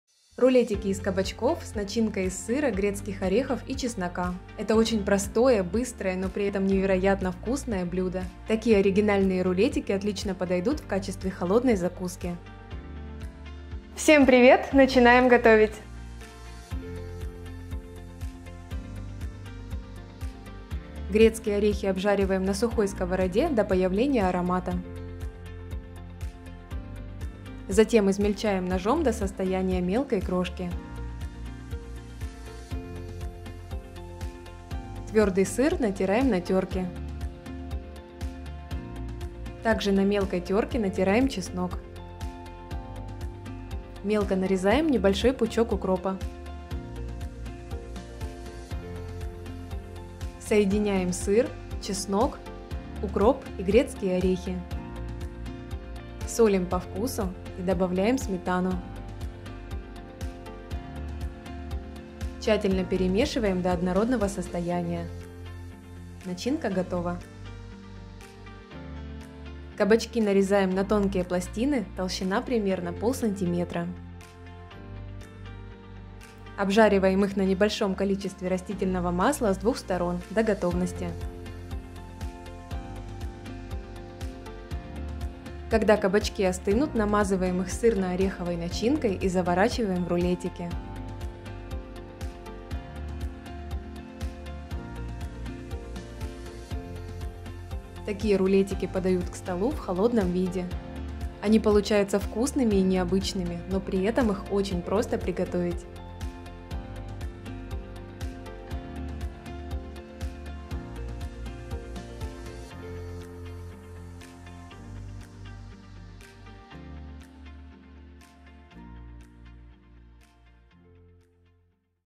РУЛЕТИКИ из КАБАЧКОВ [с сыром, грецкими орехами и чесноком] - ОРИГИНАЛЬНАЯ холодная закуска
Вкусные, нежные, ароматные и такие простые в приготовлении рулетики из кабачков.
Это идеальная холодная закуска к праздничному столу или на каждый день.
Зелень можно использовать любую - укроп, петрушку, кинзу или зелёный лук.

Ингредиенты:
- кабачки небольшие - 3 шт.
- сыр - 80 гр.;
- грецкие орехи - 50 гр.;
- укроп - небольшой пучок;

Еда, готовься!